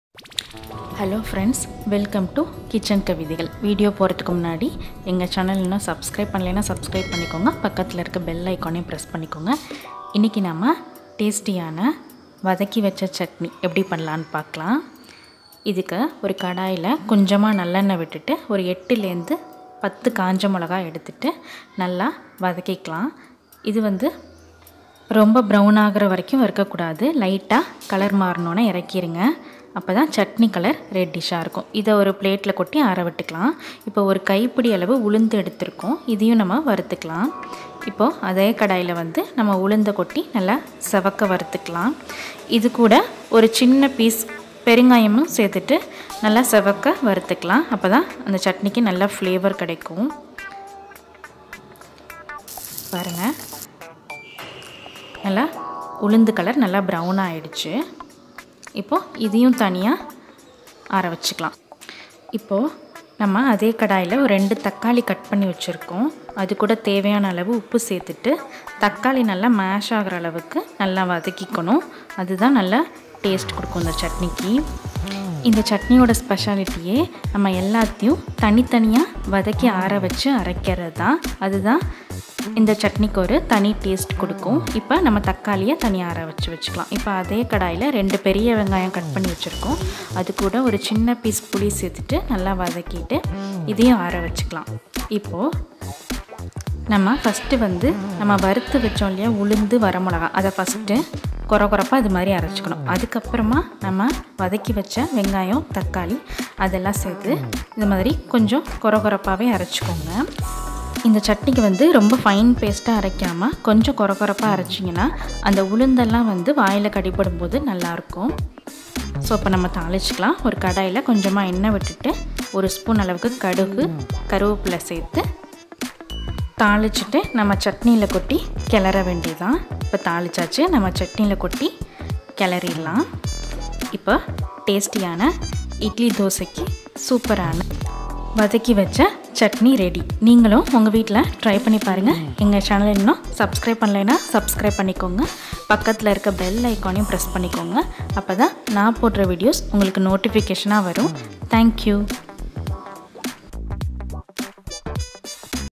வதக்கி வச்ச சட்னி/Onion and tomato chutney/இட்லி தோசைக்கு சூப்பரான சட்னி
Hi friends.. please try this tasty chutney.. which goes very well with idli and dosa.